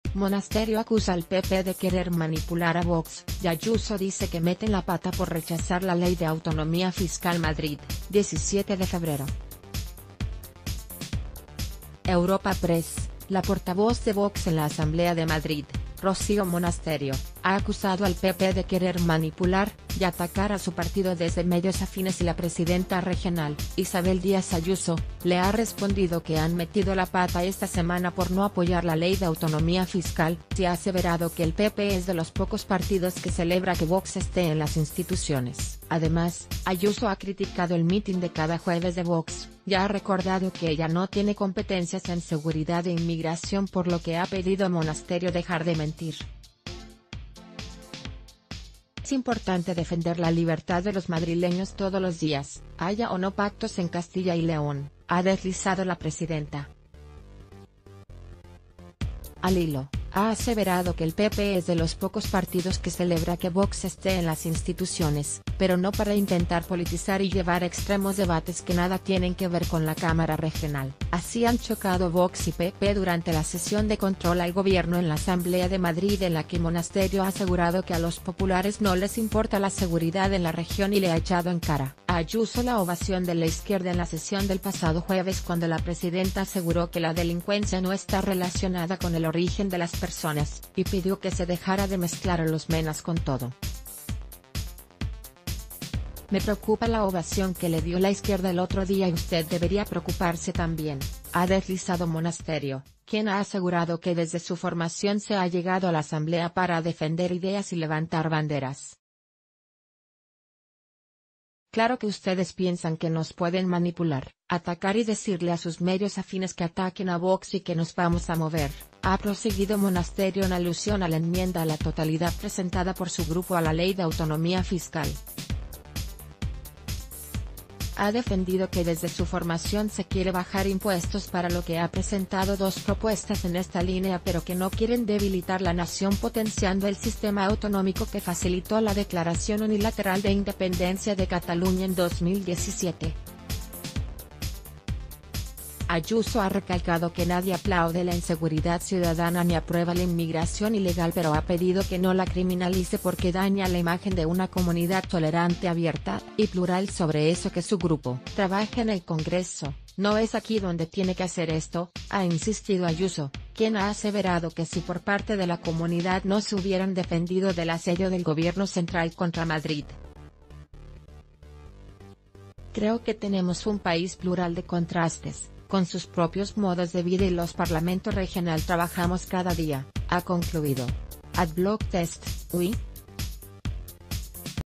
Ayuso recuerda que el PP es "de los pocos partidos" que celebra que Vox esté en las instituciones
Monasterio acusa al PP de quer...